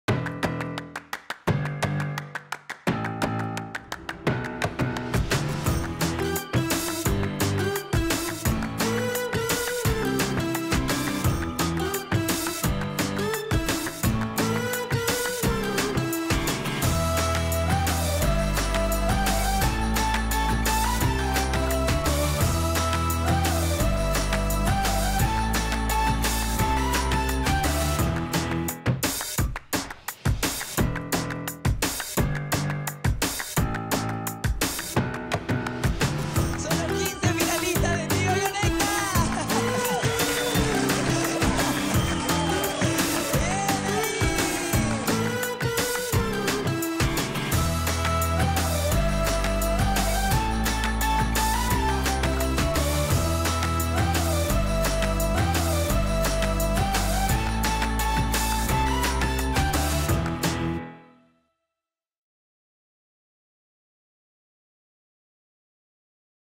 Premios Tigo Conecta 2018
Tres ideas que marcarán una diferencia en la manera de hacer negocios en Paraguay. Este fue el proceso que vivimos con los ganadores de los PremiosTigoConecta 2018